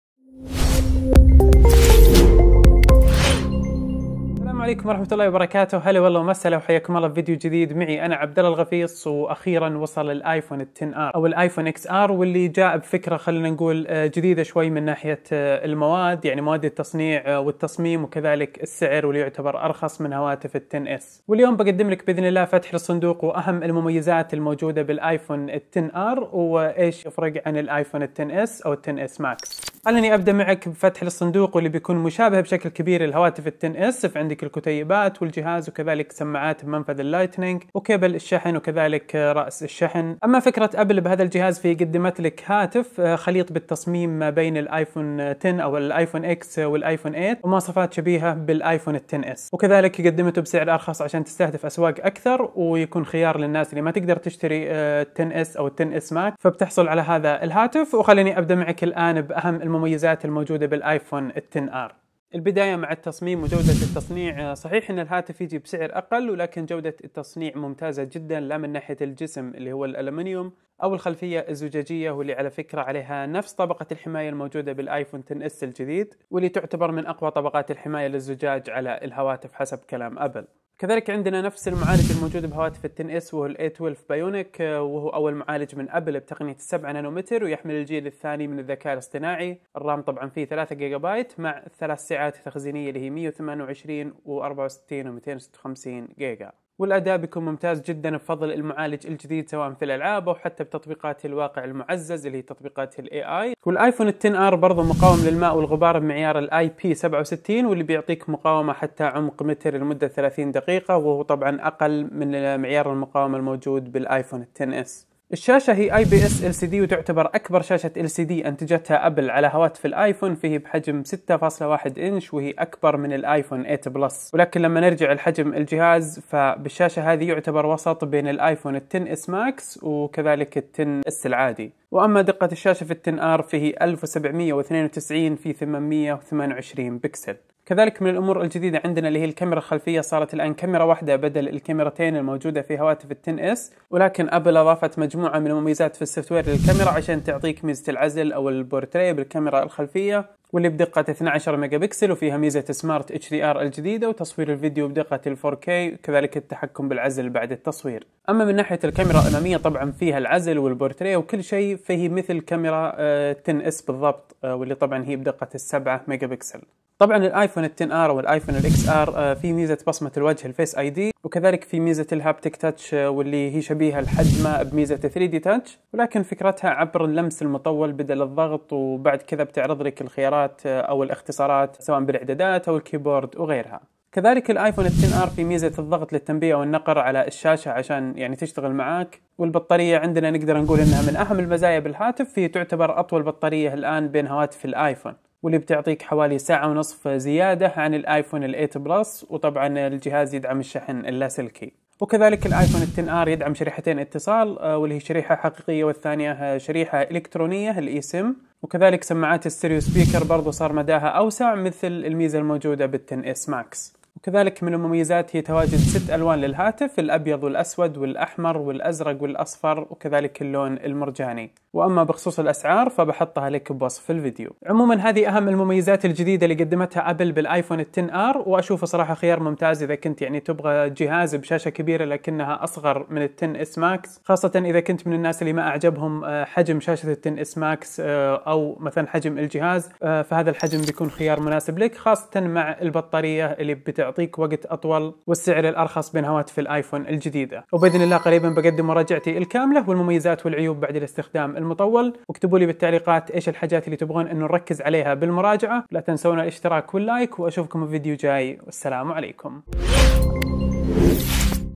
فتح صندوق واستعراض مميزات iPhone XR والفروقات عن iPhone XS
فيديو فتح صندوق واستعراض أهم مميزات وجديد Apple - أبل - أيفون اكس آر - iPhone XR - ، وتوضيح أهم الفروقات بين iPhone XR و الأيفون اكس اس - iPhone XS - .

* أسعار ايفون اكس ار - iPhone XR - :
- نسخة 64GB بسعر : 3299 ريال = 880 دولار .
- نسخة 128GB بسعر : 3499 ريال = 933 دولار .
- نسخة 256GB بسعر : 3949 ريال = 1053 دولار .

* فيديو مراجعة أيفون اكس آر - iPhone XR - .
( قريباً ) .
❖ ادعم قناتنا عبر التسوق من أمازون باستخدام الرابط التالي :

* حساب عبدالله الغفيص على سناب شات :
agafes